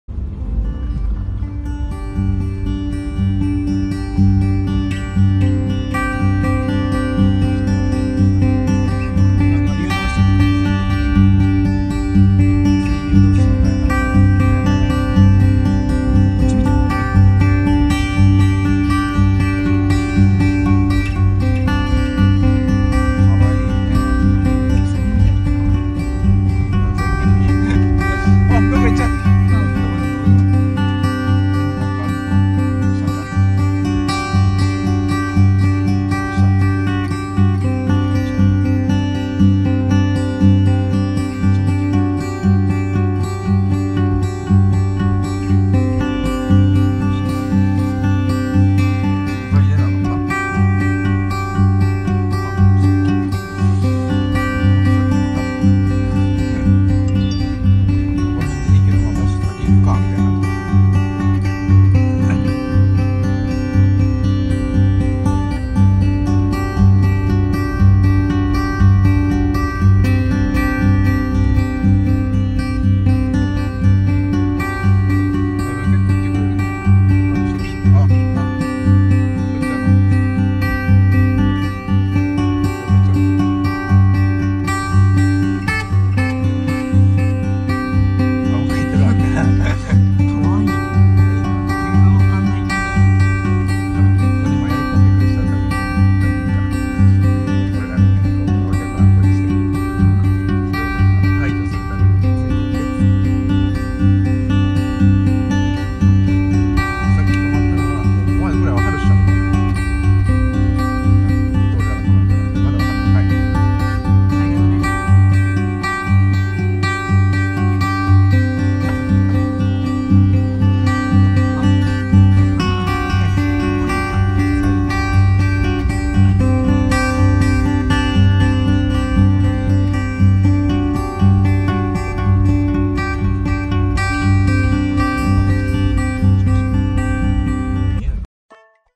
是山神化身？ 神秘灰白貓帶迷途駕駛脫困 從深山帶到去國道
前幾天，日本一名網友開車到山中「探險」。他準備的很充分，車上裝有導航系統，但還是迷失方向，不知道該怎麼辦。正在這時，一隻灰白貓突然出現。牠好像知道網友陷入困境，來到車子前面帶路，看起來非常有靈性。